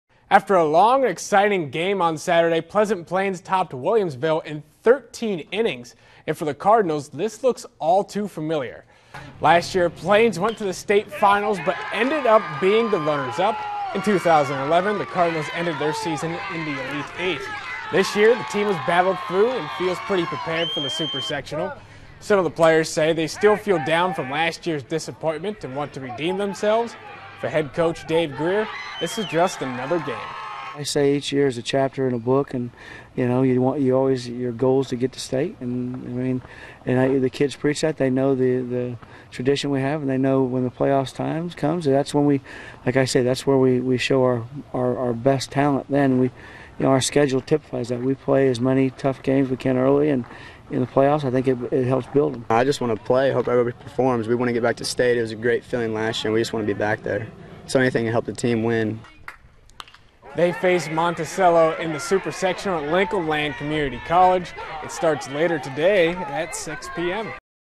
Pleasant Plains Prepares for Super-Sectional 6-1-2
Pleasant Plains baseball prepares for it's super-sectional game against Monticello.